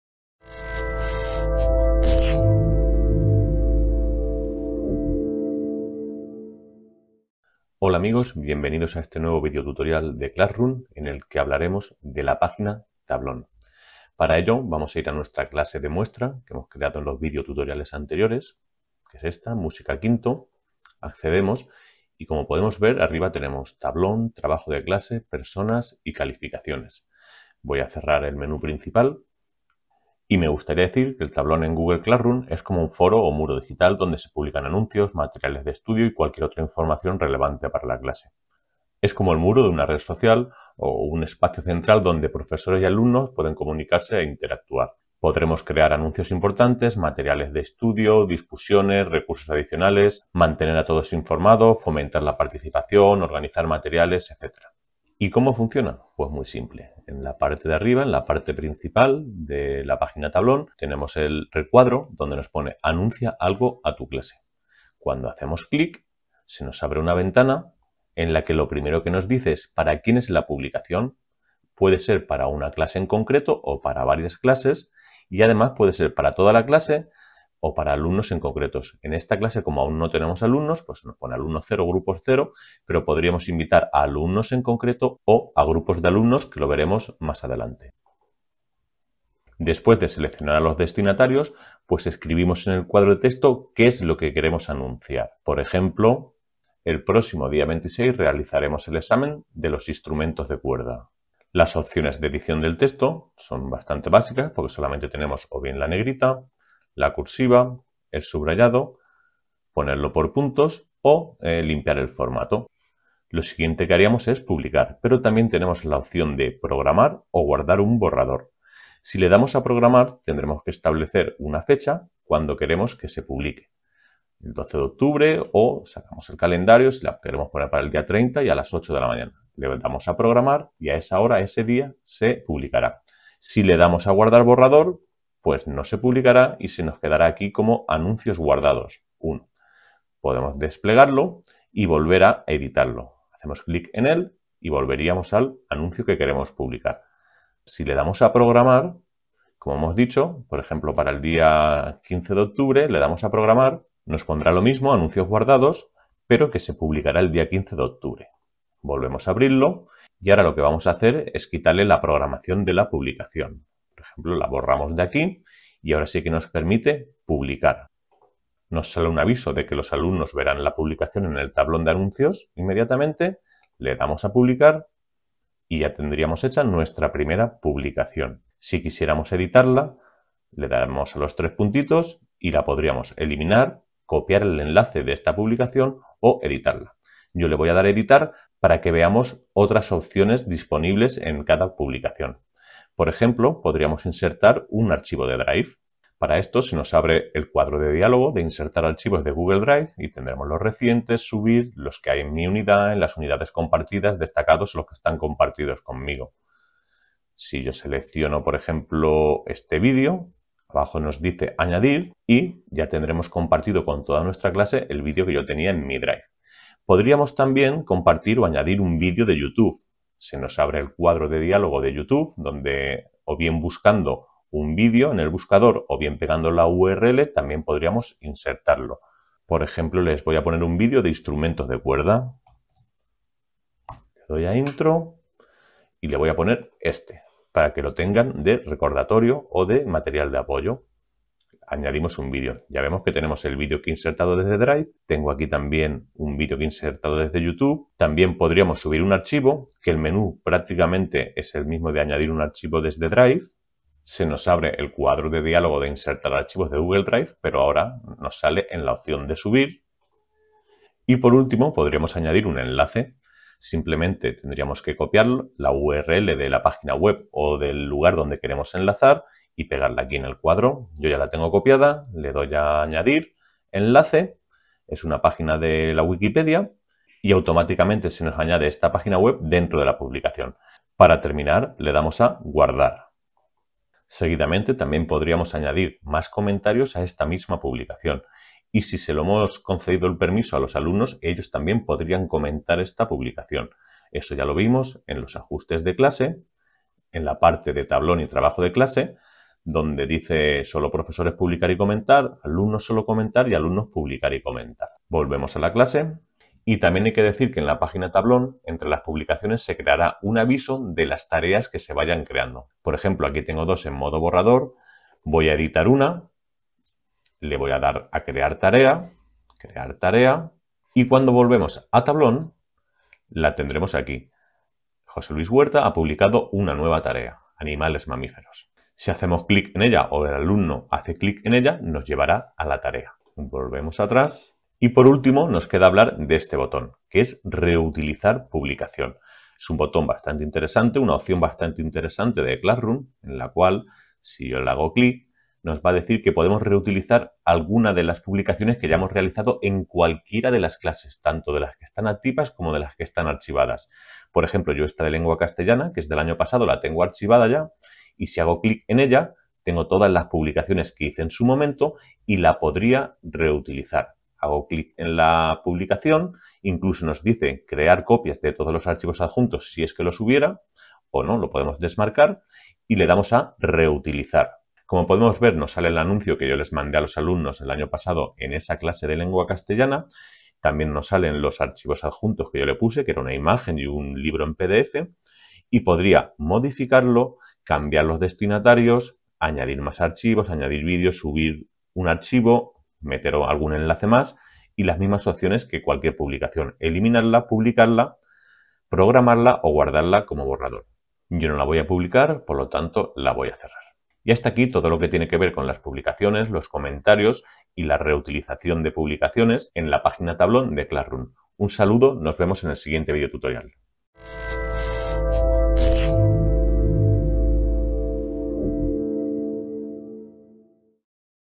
07 Classroom: publicar en tablón
En este vídeo tutorial aprenderás a crear publicaciones en la página "Tablón" de Classroom.